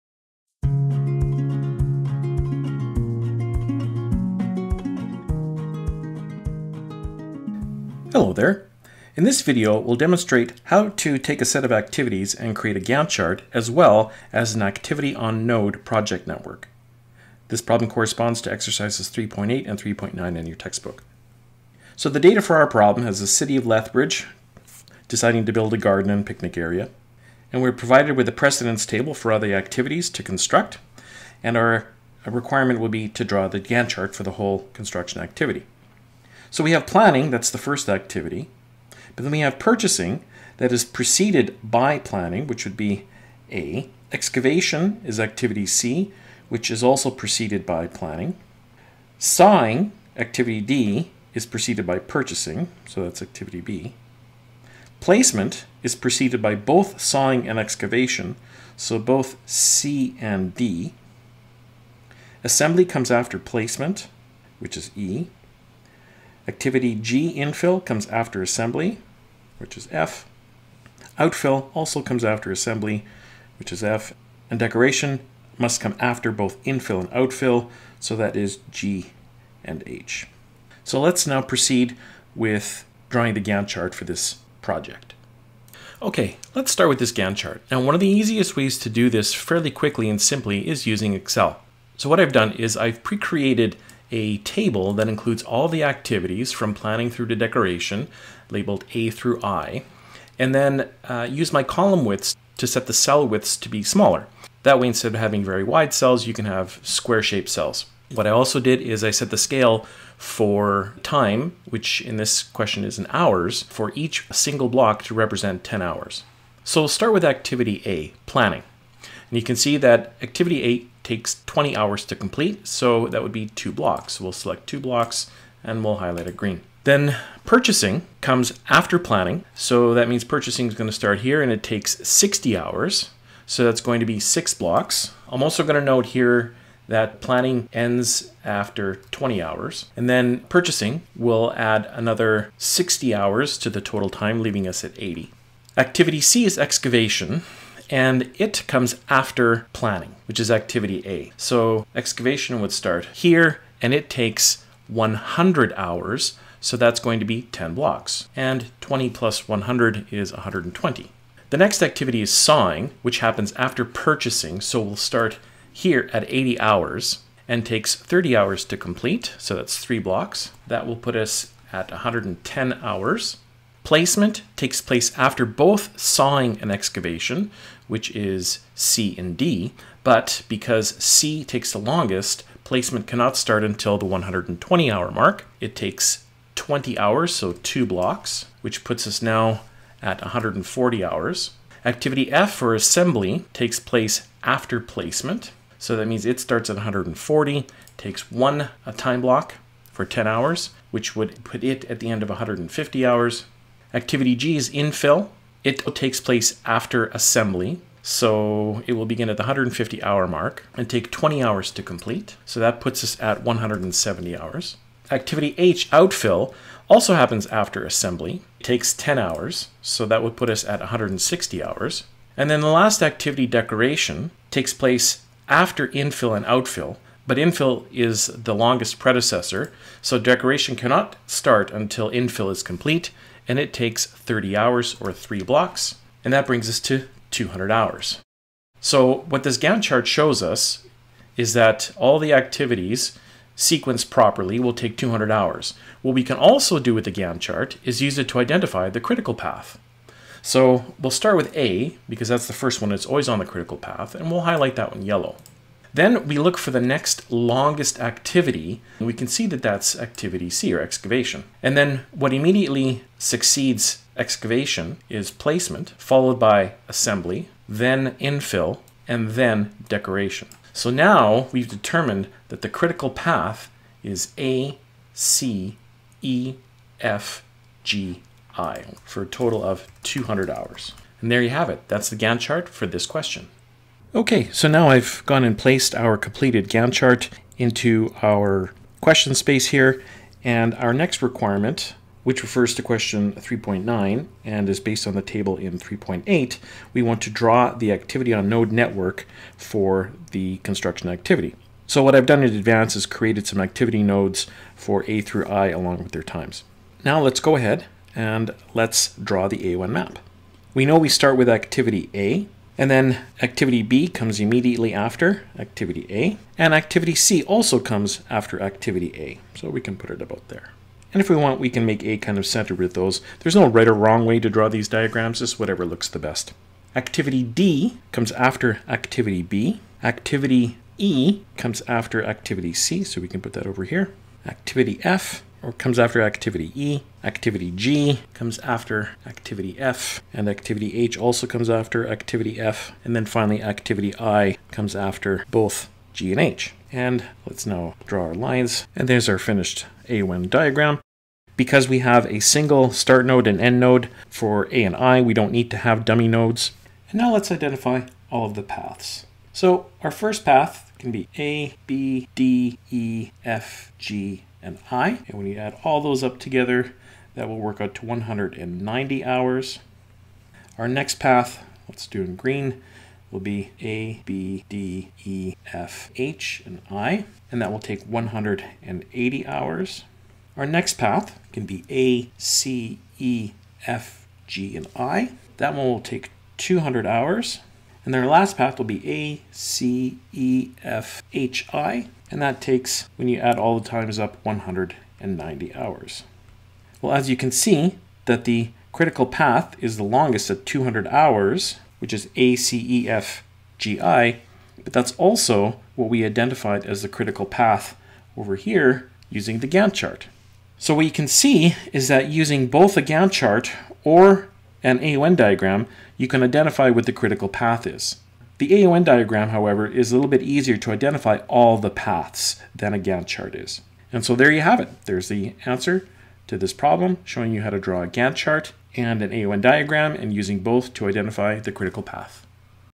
Project Management: Gantt Chart and AON Project Network IV
This video demonstrates how to draw a Gantt Chart and a PERT/CPM AON (Activity-on-Node) project network for a project with 9 tasks.